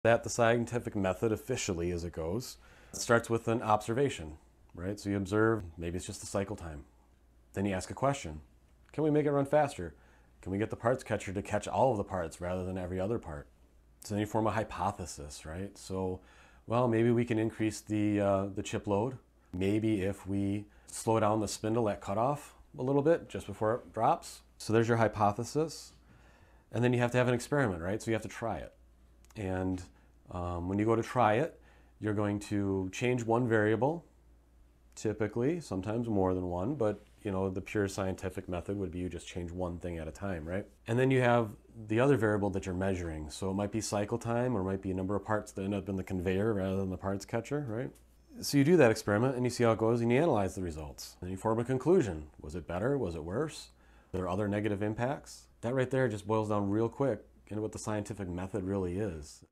The Scientific Method in Machining
A clip from our live stream series "Machining Is...". Tim talks about what the Scientific Method is, and how the Scientific Method could be using in the machining world!

Do you use the Scientific Method in your machining day to day actions?